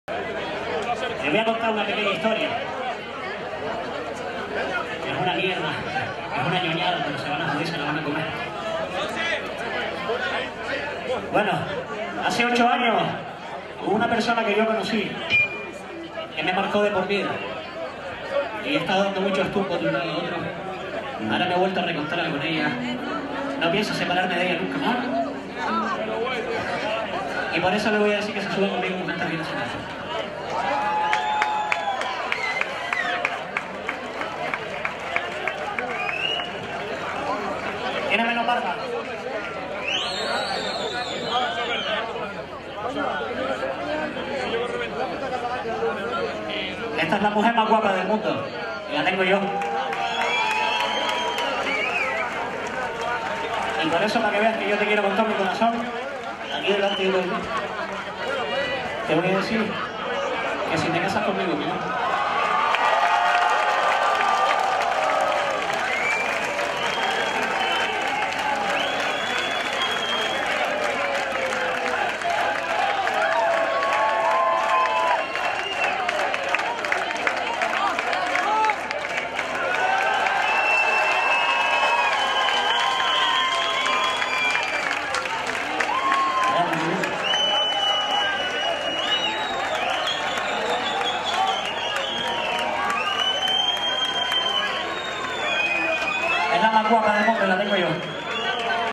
Pedida de mano en el Goymar Fest 2018
Esta edición del Goymar Fest trajo sorpresa, la pedida de mano en el Goymar Fest del cantante de An Endless Path.  Y dijo si!
Enhorabuena pareja.